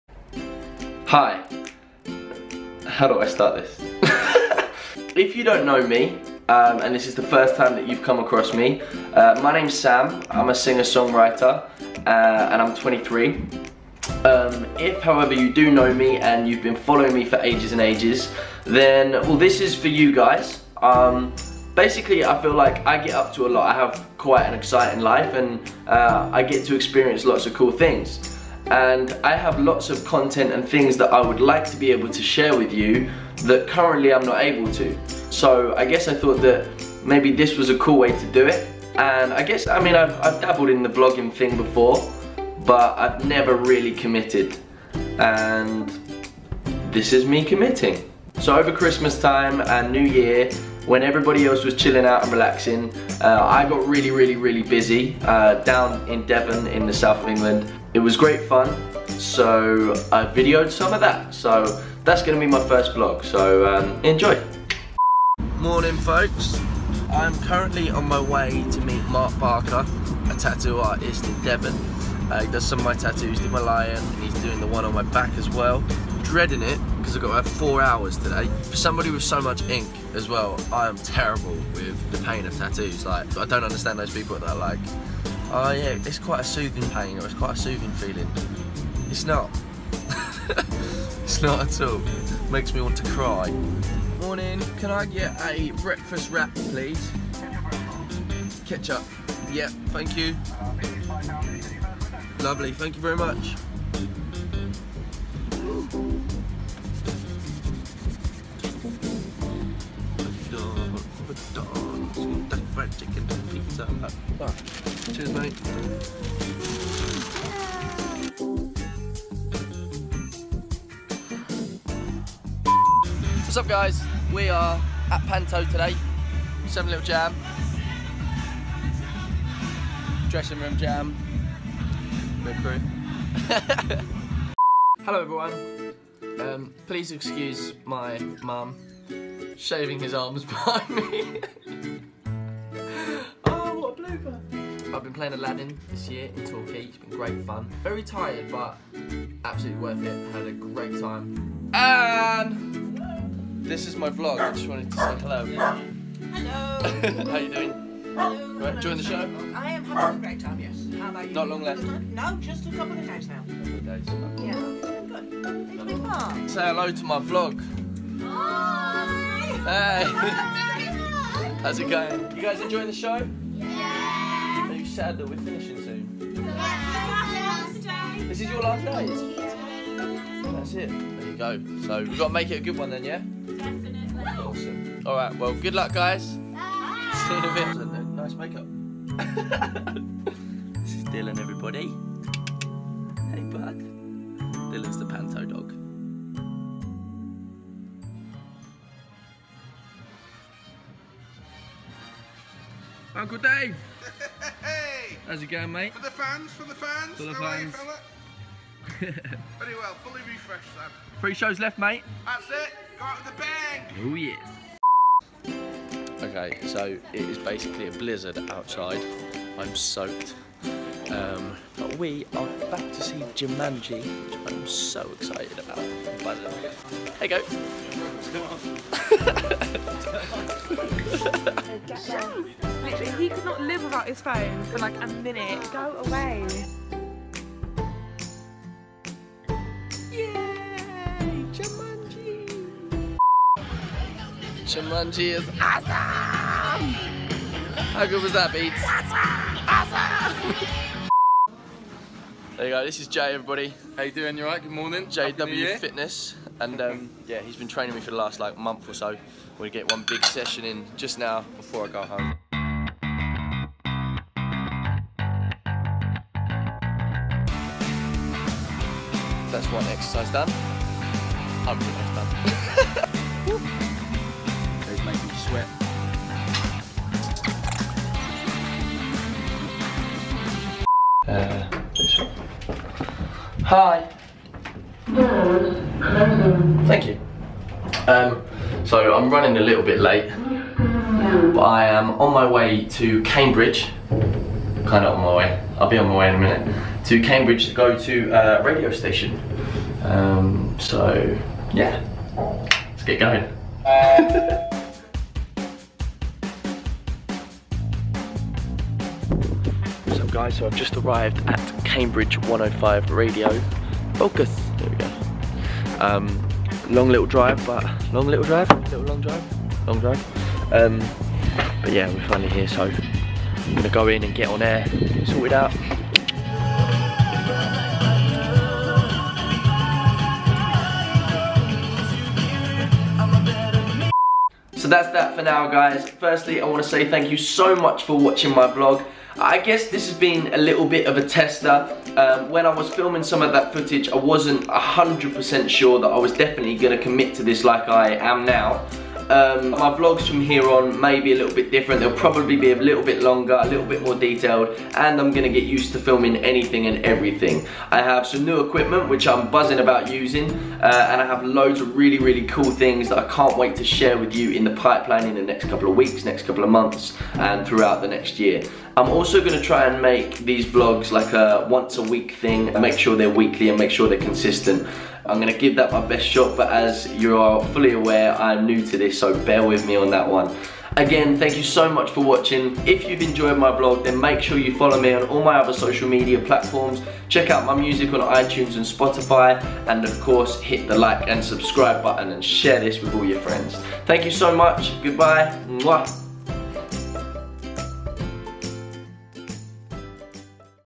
SHOULD I START TO VLOG?
Whats up ladies and gents!

So I thought I'd test the waters and see what all this vlogging hype is about, only I kept forgetting to vlog! OOPS.

Anyway, here is my 'SHOULD I START TO VLOG?" vlog with some bits and bobs that I put together from the end of 2017.